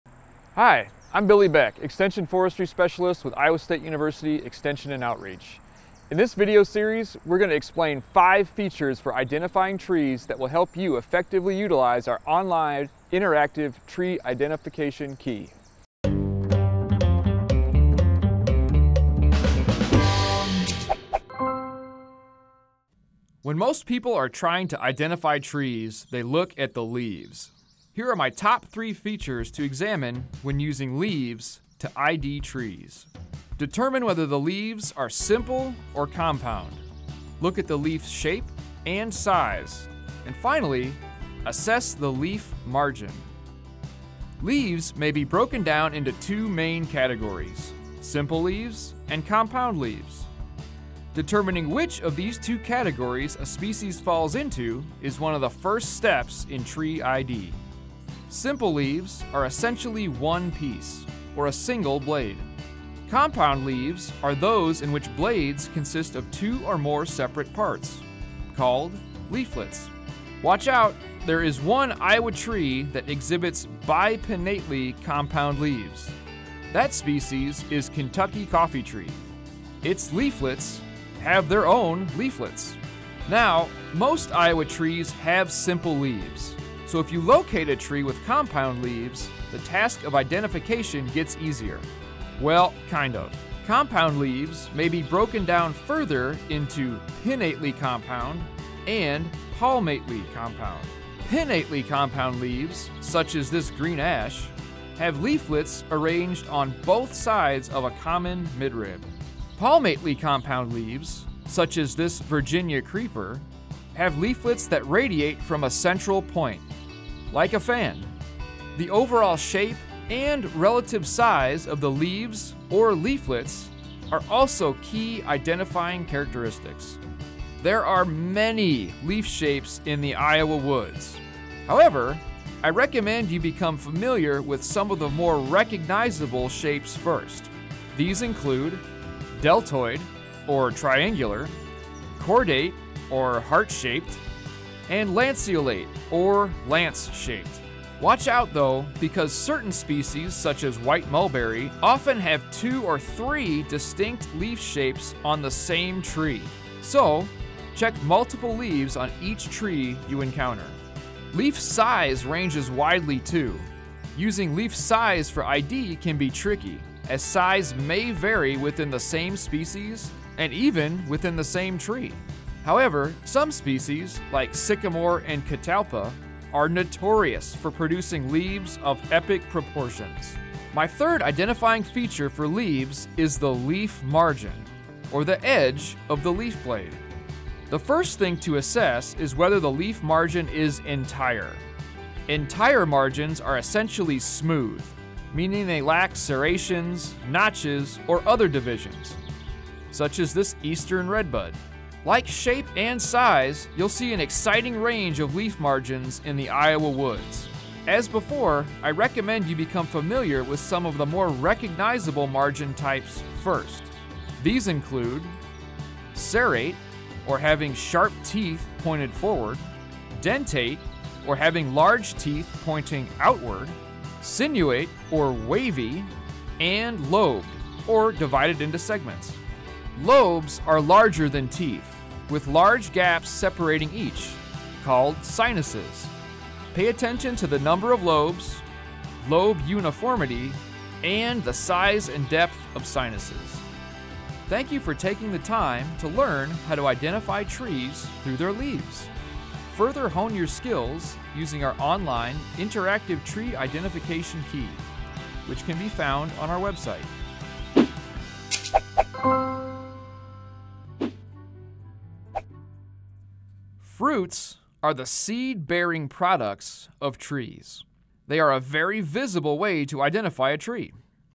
How to Use Leaves to Identify Trees
When most people are trying to identify trees, they look at the leaves. Billy Beck, Iowa State University assistant professor and extension forestry specialist, shares his top 3 leaf features to examine – which will help you use ISU Extension and Outreach’s online, interactive tree identification key.